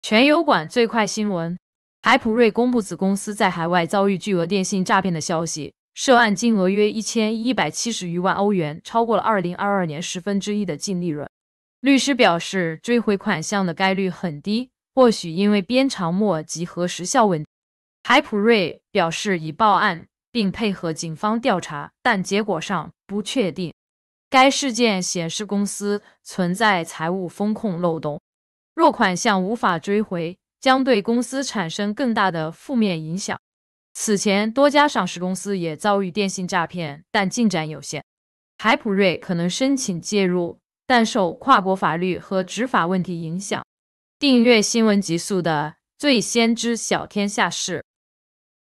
中国新闻01月15日08时：A股史上最大电诈案！深圳一药企国外被骗近1亿 损失前三季度净利一大半
海普瑞（002399.SZ）公布子公司在海外遭遇巨额电信诈骗的消息，涉案金额约1170余万欧元，超过了2022年十分之一的净利润。律师表示，追回款项的概率很低，或许因为鞭长莫及和时效问题。海普瑞表示已报案并配合警方调查，但结果尚不确定。该事件显示公司存在财务风控漏洞。若款项无法追回，将对公司产生更大的负面影响。此前，多家上市公司也遭遇电信诈骗，但进展有限。海普瑞可能申请介入，但受跨国法律和执法问题影响。